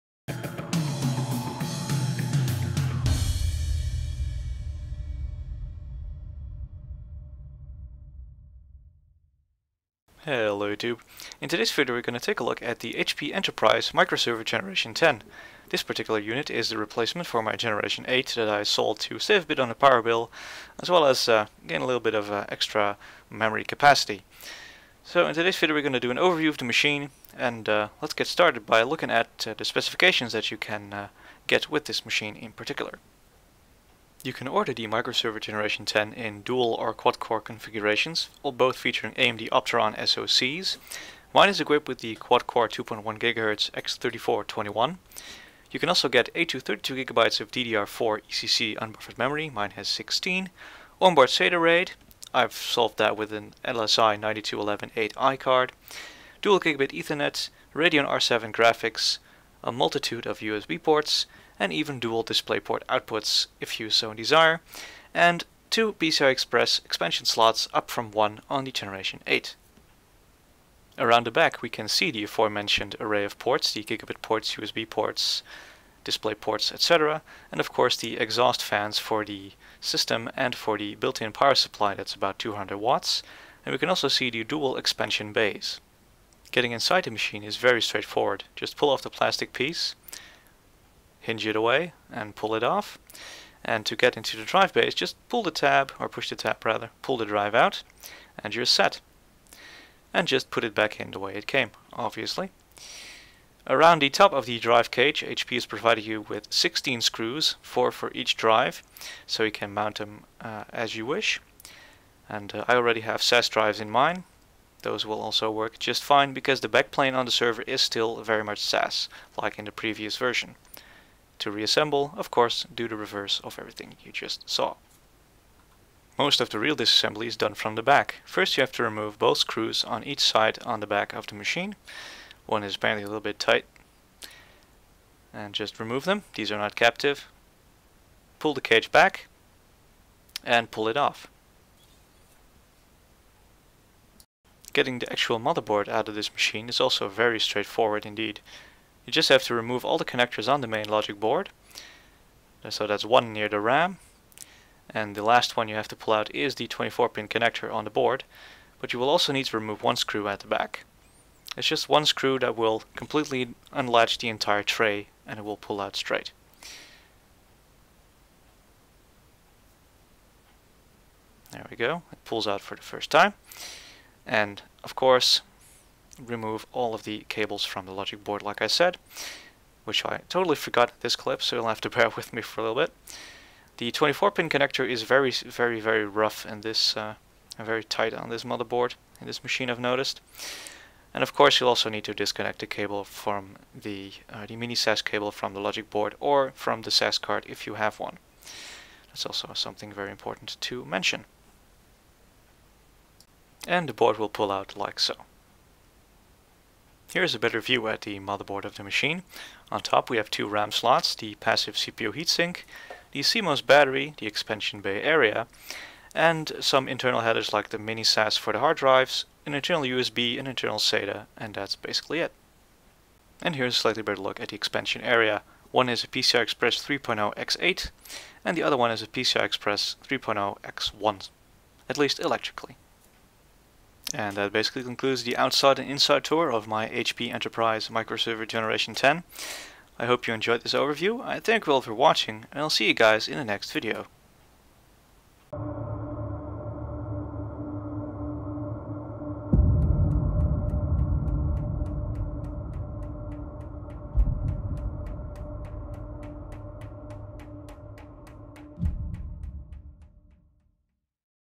HP Enterprise - Microserver Gen10 - Overview
A short overview on HPE's latest generation of their entry level server. In this video, we will take a quick look at the specs and how to get inside the machine.

- For updates and general stuff between videos, visit my